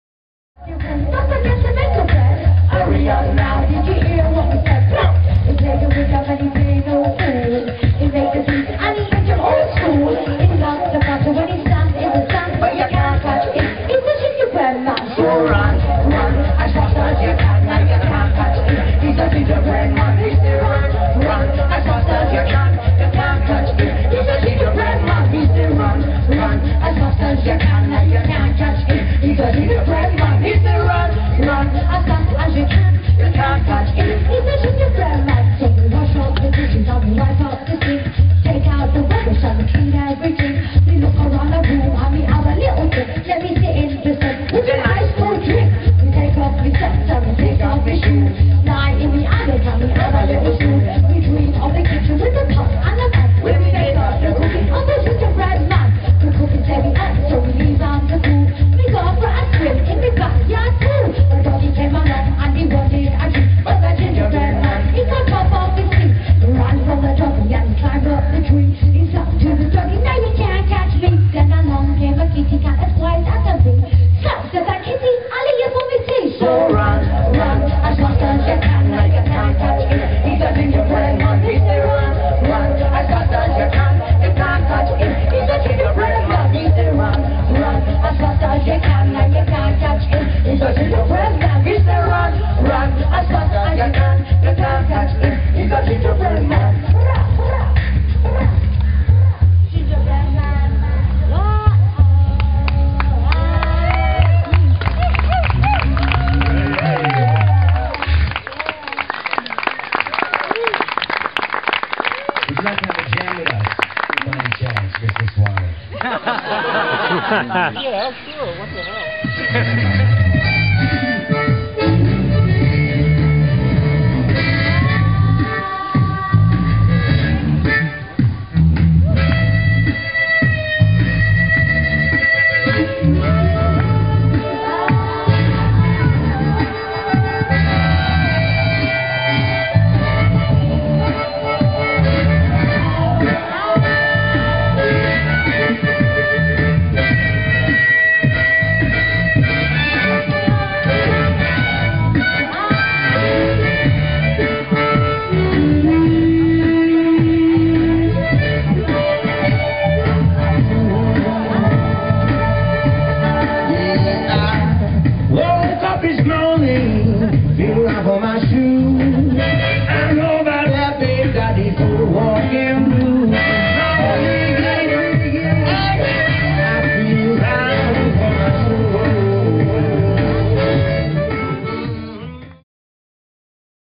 Dub FX in India with Swami Shankarananda
Dub FX visits a meditation retreat in Ganeshpuri, near Bombay and performs a duet with Mahamandaleshwar Swami Shankarananda.